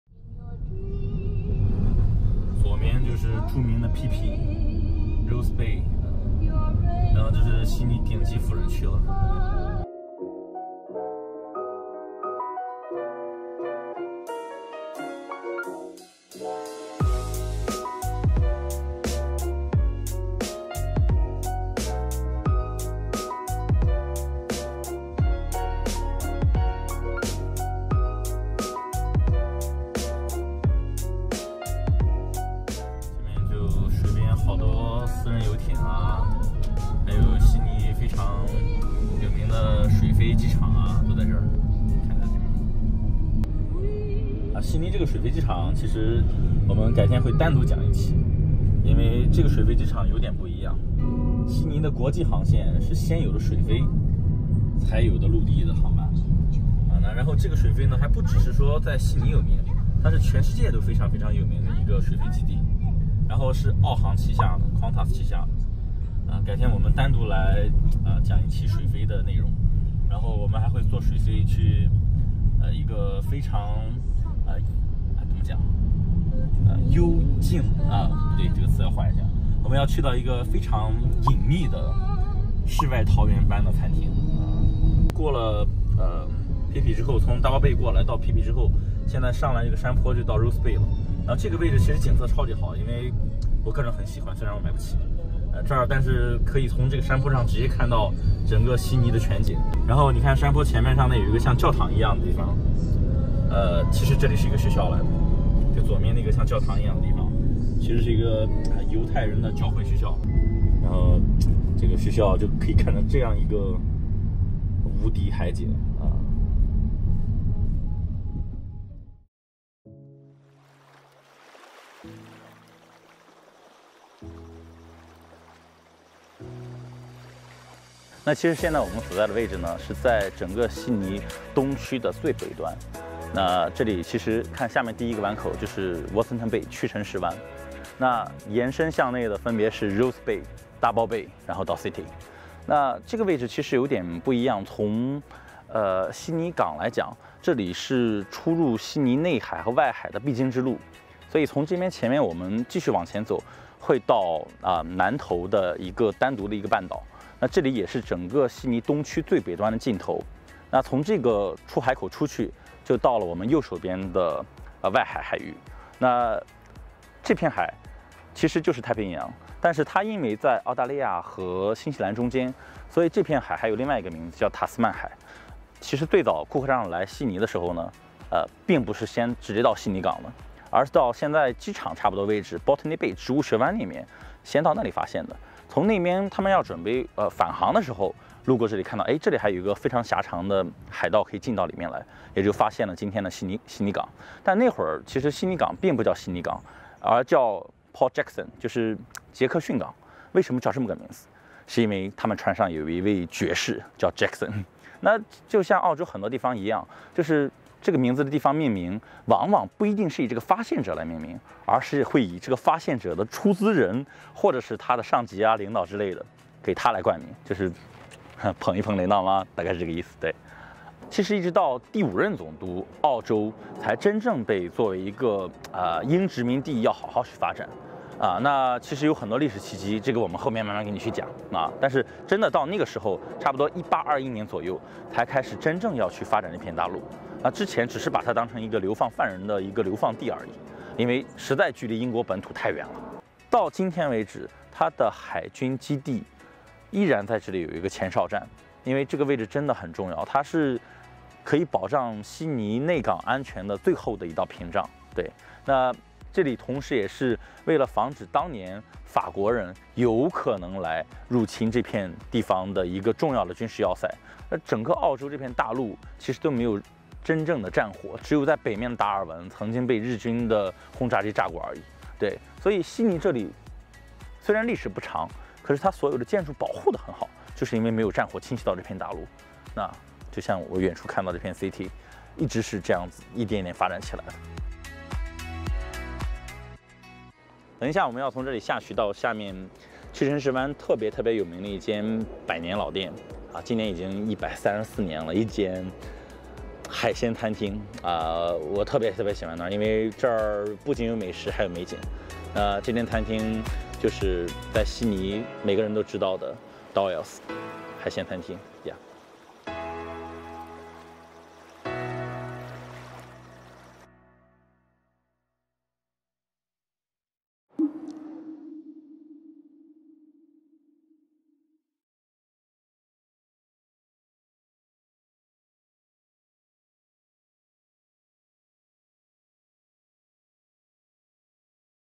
【澳洲旅行 Vlog 03】悉尼 "富人区" 在哪里 | Watsons Bay 屈臣氏湾 自杀崖 南头 | 澳大利亚 悉尼东区 | 旅拍 Vlog
Watsons Bay 屈臣氏湾
是悉尼东郊的一个海港，位于澳大利亚新南威尔士州。
屈臣氏湾位于悉尼中央商务区东北11公里处，坐落在南头半岛的尽头，其名字来自位于杰克逊港的西侧的避风海湾和锚地。享有海港至悉尼海港大桥的景致，峡谷是东侧的海洋悬崖。